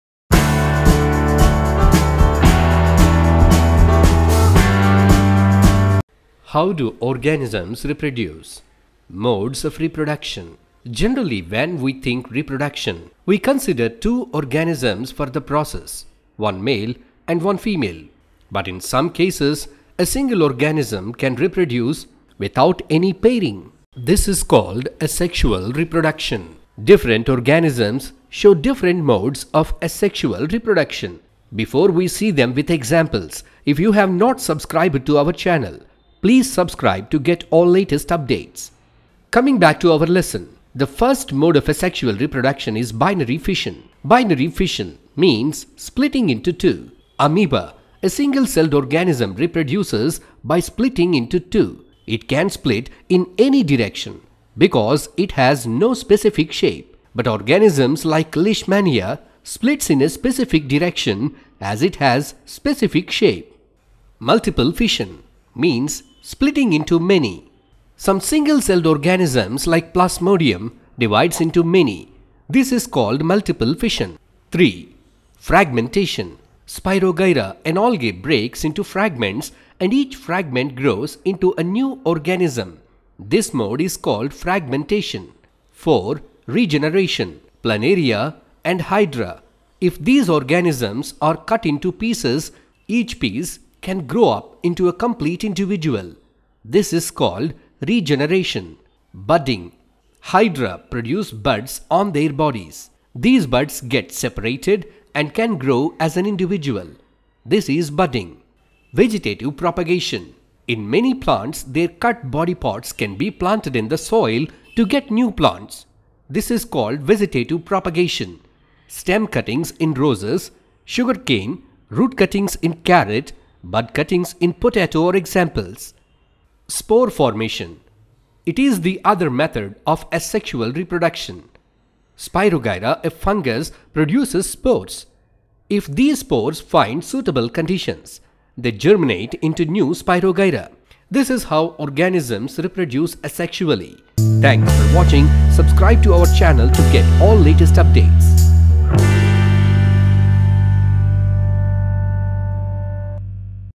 How do organisms reproduce : 10th Biology : CBSE Syllabus : ncert class 10 : X Science
How do organisms reproduce? | CBSE Class 10 Science Syllabus | Biology | NCERT 10th Class | Best Animated video Lectures

This video explains  CBSE Class 10th Biology " How do organisms reproduce " in the lessons How do organisms reproduce ? in an easy and compact way with rich animations. This video helps you to understand the concept with great ease and clarity.